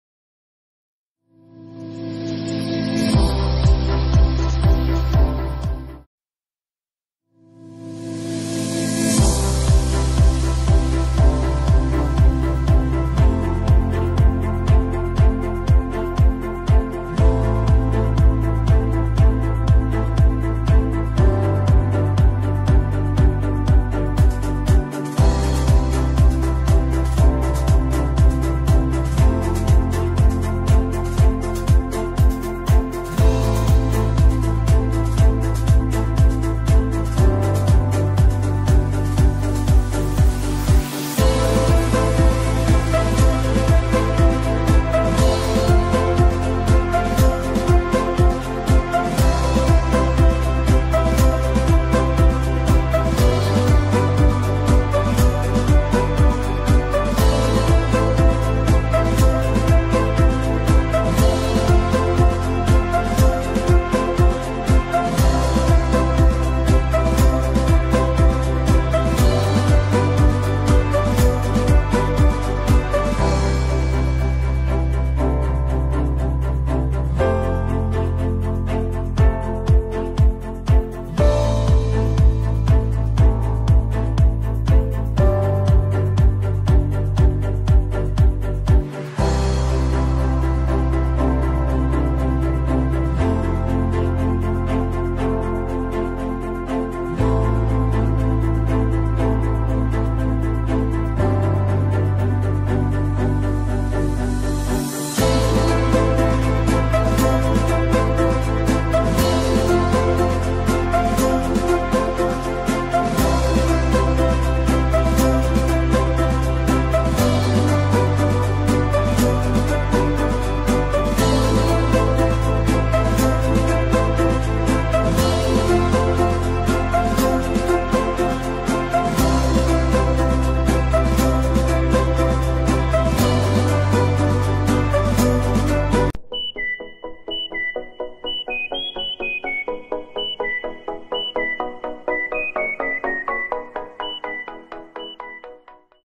El negocio que les hará competencia a los famosos ‘corrientazos’ en bogotá
El nombre del negocio es El Sello del Chef, y tiene un modelo de buffet que incluye más de 25 menús.